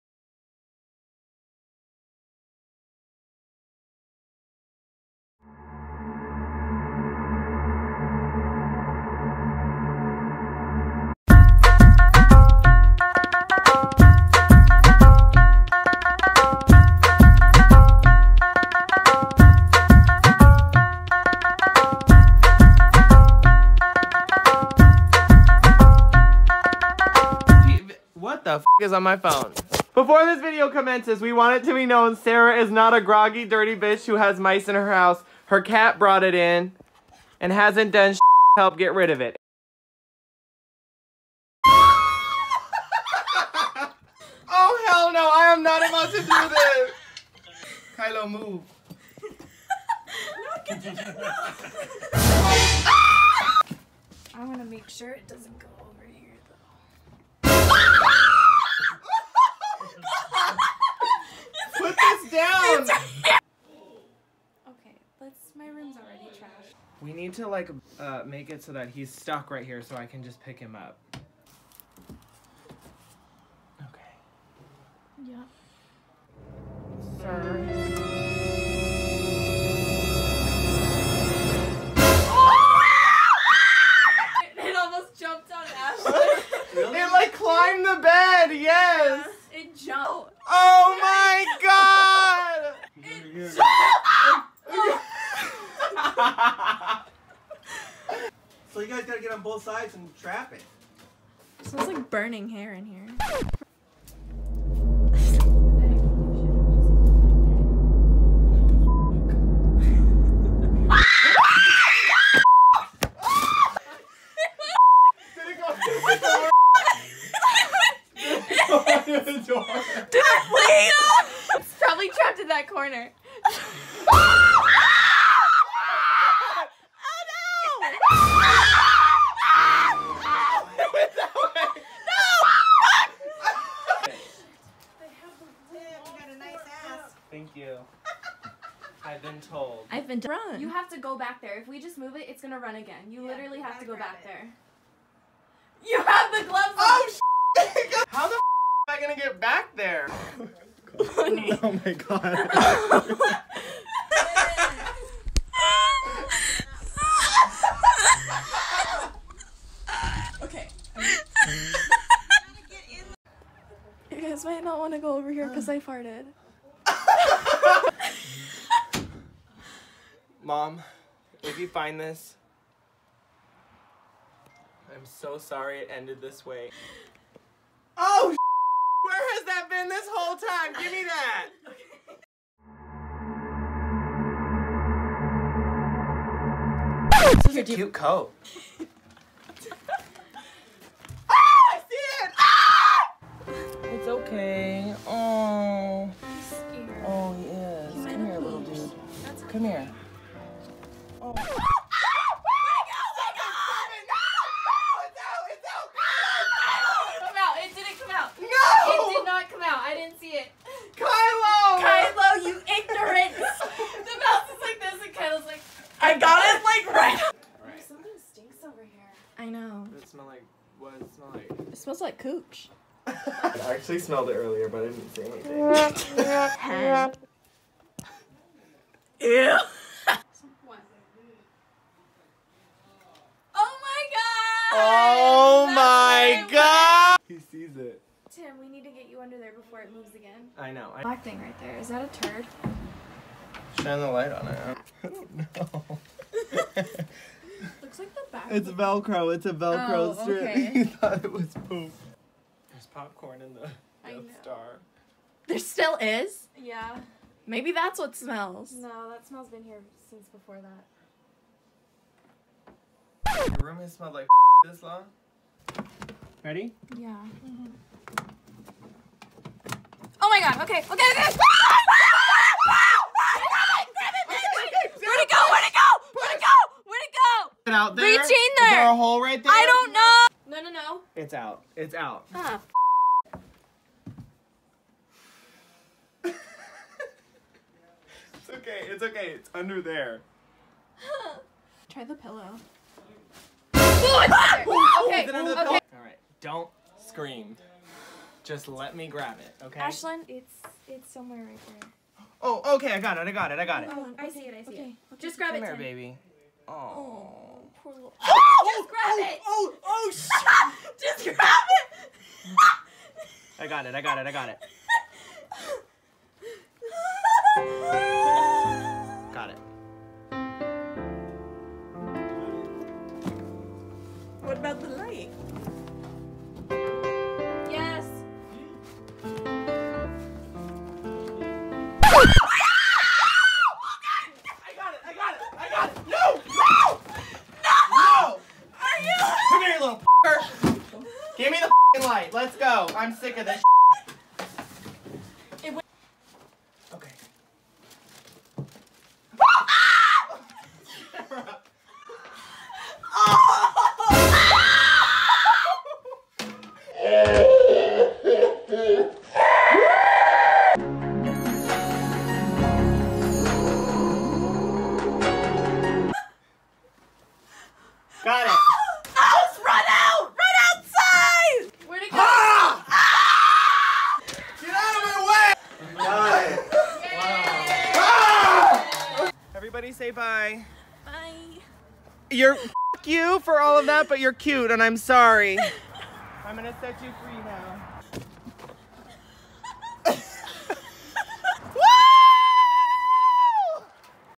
RAT HUNT!
Sarah's cat brought a mouse into her room...

Hey y'all! I'm now under my stage name, My Name Is Chapel.